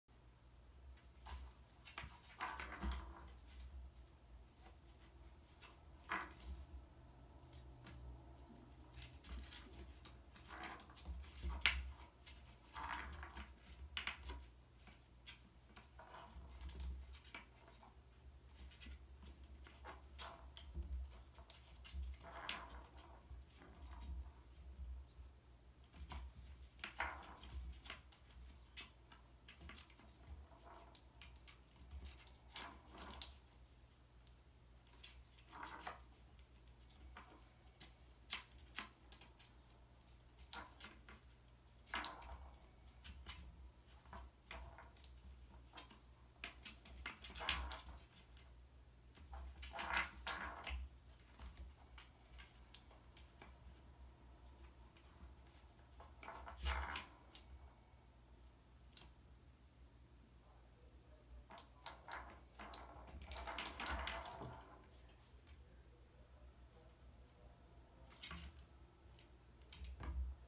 Daisy and Cooper playing marbles
Daisy and Cooper love playing with the marbles embedded in the base of one of their scratching posts. Cooper can get a little possessive, but Daisy doesn't seem to mind.